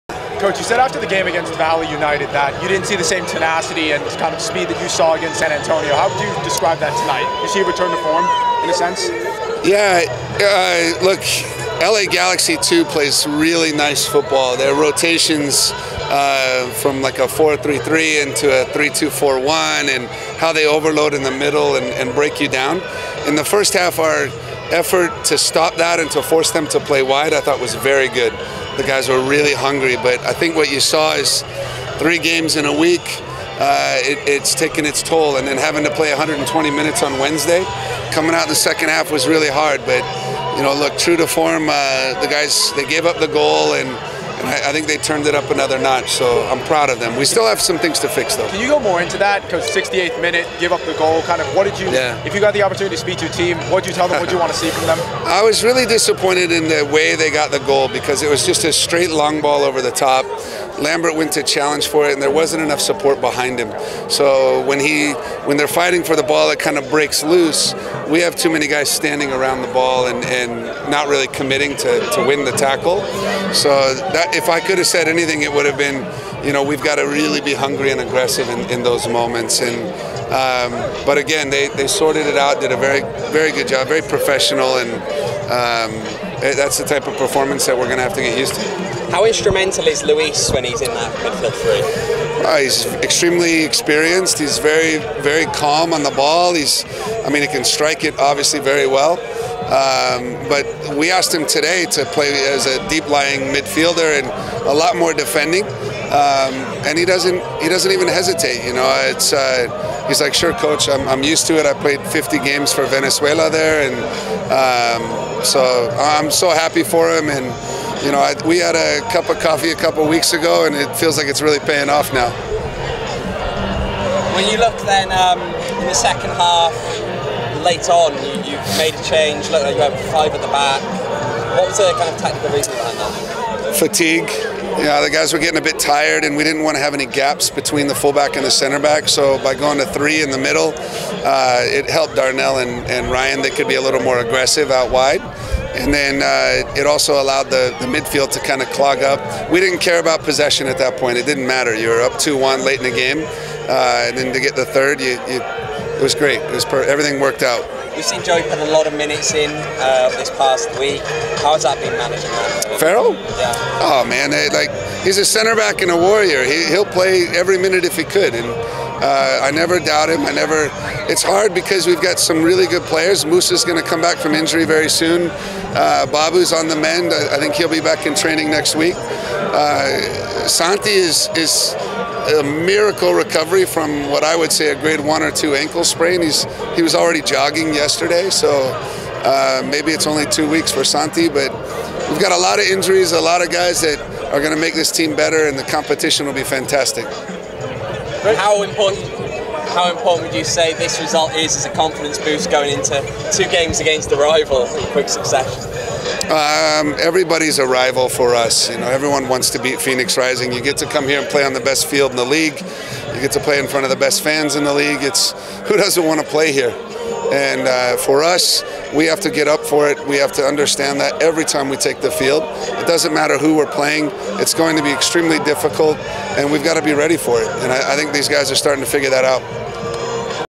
Postgame: Rick Schantz 4.9.22
Head Coach Rick Schantz speaks to the media following Phoenix Rising’s 3-1 win against LA Galaxy II.